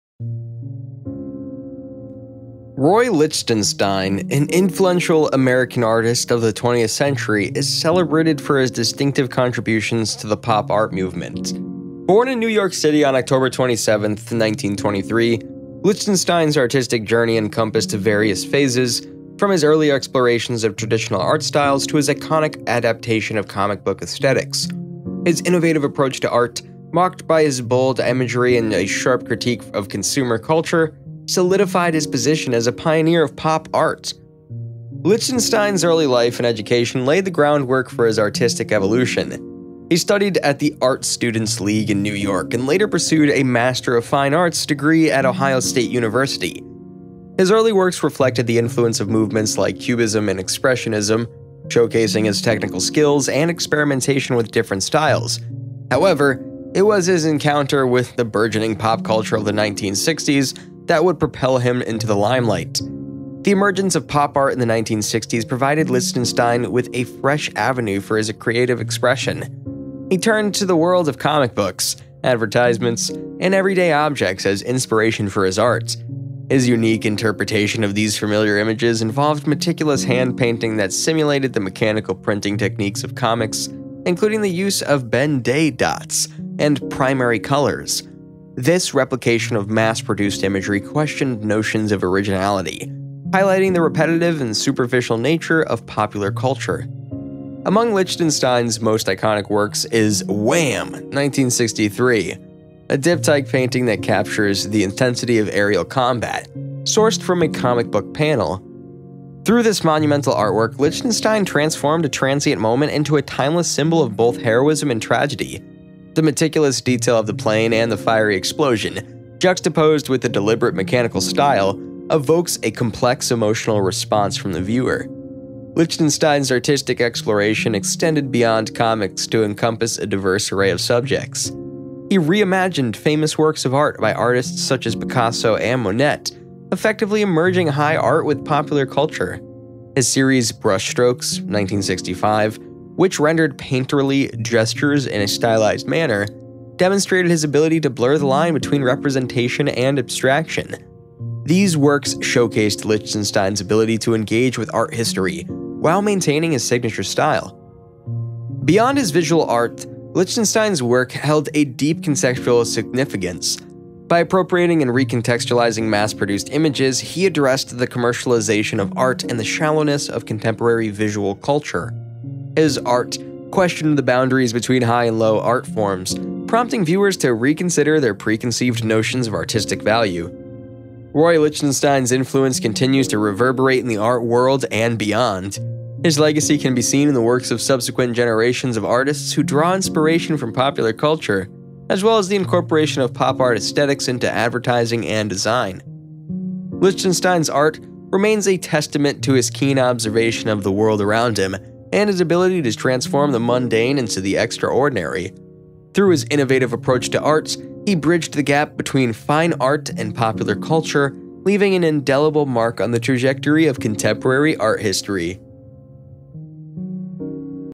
famous artist Roy Lichtenstein
Roy Lichtenstein, an influential American artist of the 20th century, is celebrated for his distinctive contributions to the pop art movement. Born in New York City on October 27, 1923, Lichtenstein's artistic journey encompassed various phases, from his early explorations of traditional art styles to his iconic adaptation of comic book aesthetics. His innovative approach to art, marked by bold imagery and a sharp critique of consumer culture, solidified his position as a pioneer of pop art.
Lichtenstein's early life and education laid the groundwork for his artistic evolution. He studied at the Art Students League in New York and later pursued a Master of Fine Arts degree at Ohio State University. His early works reflected the influence of movements like Cubism and Expressionism, showcasing his technical skill and experimentation with different styles. However, it was his encounter with the burgeoning pop culture of the 1960s that would propel him into the limelight.
The emergence of pop art in the 1960s provided Lichtenstein with a fresh avenue for his creative expression. He turned to the world of comic books, advertisements, and everyday objects as inspiration for his art. His unique interpretation of these familiar images involved meticulous hand-painting that simulated the mechanical printing techniques of comics, including the use of Ben-Day dots and primary colors. This replication of mass-produced imagery questioned notions of originality, highlighting the repetitive and superficial nature of popular culture.
Among Lichtenstein's most iconic works is "Whaam!" (1963), a diptych painting that captures the intensity of aerial combat, sourced from a comic book panel. Through this monumental artwork, Lichtenstein transformed a transient moment into a timeless symbol of both heroism and tragedy. The meticulous detail of the plane and the fiery explosion, juxtaposed with the deliberate mechanical style, evokes a complex emotional response from the viewer.
Lichtenstein's artistic exploration extended beyond comics to encompass a diverse array of subjects. He reimagined famous works of art by artists such as Picasso and Monet, effectively merging high art with popular culture. His series "Brushstrokes" (1965), which rendered painterly gestures in a stylized manner, demonstrated his ability to blur the line between representation and abstraction. These works showcased Lichtenstein's ability to engage with art history while maintaining his signature style.
Beyond his visual art, Lichtenstein's work held a deep conceptual significance. By appropriating and recontextualizing mass-produced images, he addressed the commercialization of art and the shallowness of contemporary visual culture. His art questioned the boundaries between high and low art forms, prompting viewers to reconsider their preconceived notions of artistic value.
Roy Lichtenstein's influence continues to reverberate in the art world and beyond. His legacy can be seen in the works of subsequent generations of artists who draw inspiration from popular culture, as well as in the incorporation of pop art aesthetics into advertising and
design. Lichtenstein's art remains a testament to his keen observation of the world around him and his ability to transform the mundane into the extraordinary. Through his innovative approach to art, he bridged the gap between fine art and popular culture, leaving an indelible mark on the trajectory of contemporary art history.